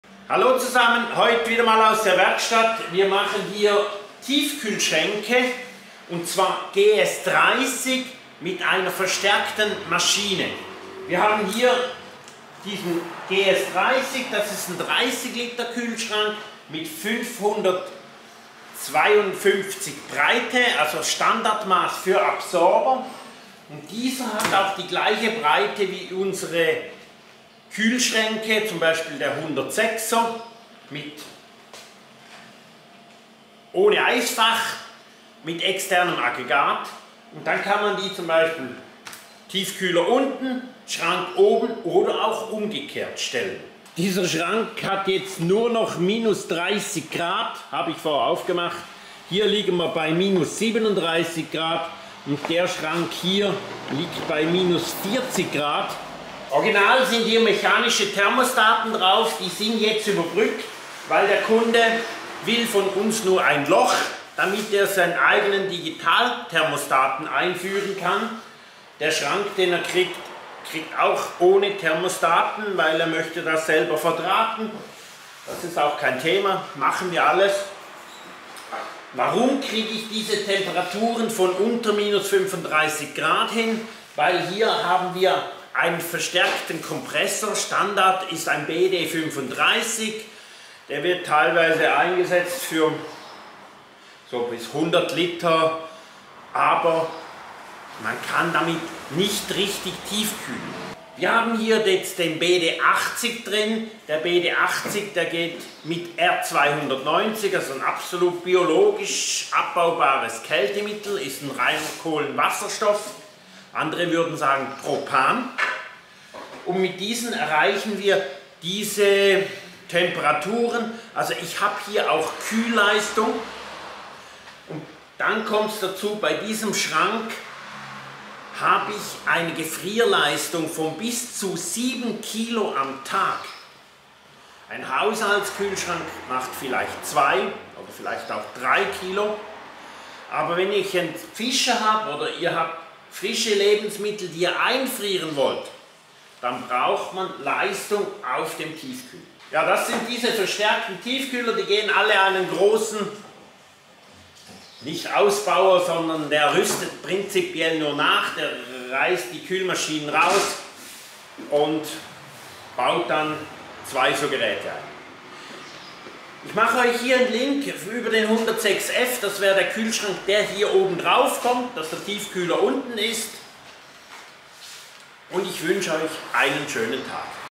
WEMO 30GS mit Kompressor SECOP BD80CN R290 für Boot und Wohnmobil Test Bericht Erfahrung
Tiefkühlschrank / Gefrierschrank mit 12 Volt